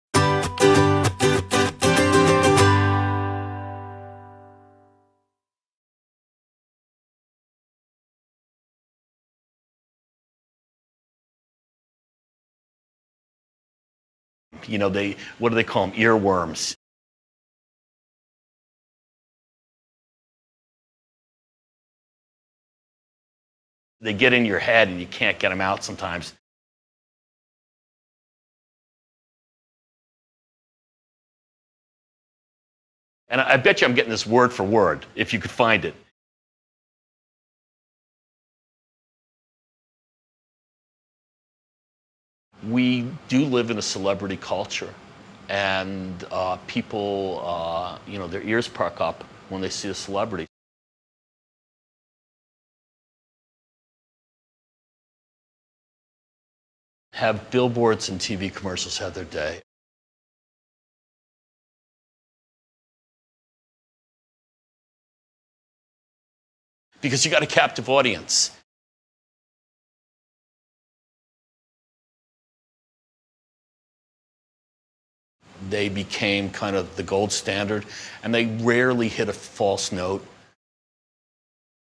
New English File Upper Intermediate Units 8&9 - Advertising / Looking at language
The interview - George Tannenbaum